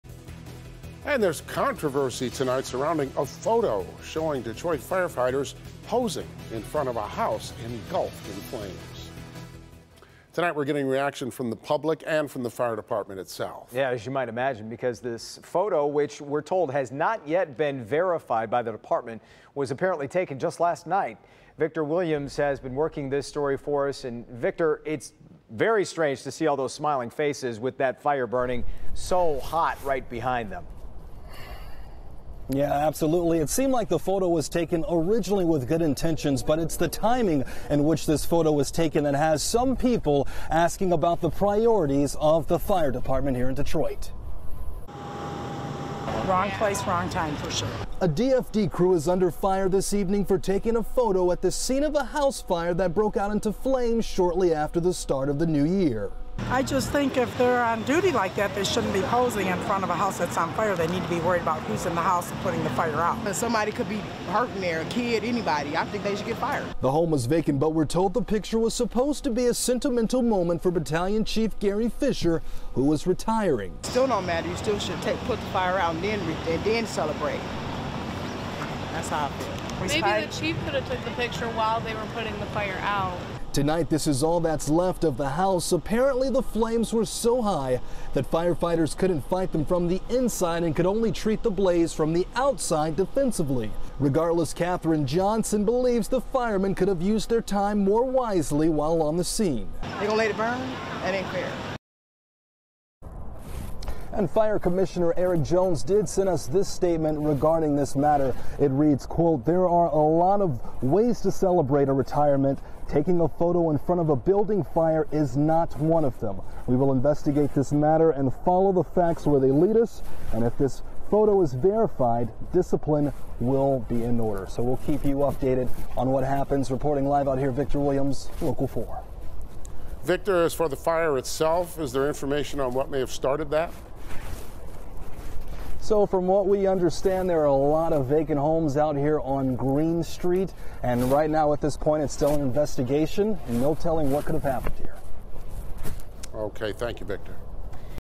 House fire photo sparks controversy in Detroit
A photo of a house fire is sparking controversy in Detroit.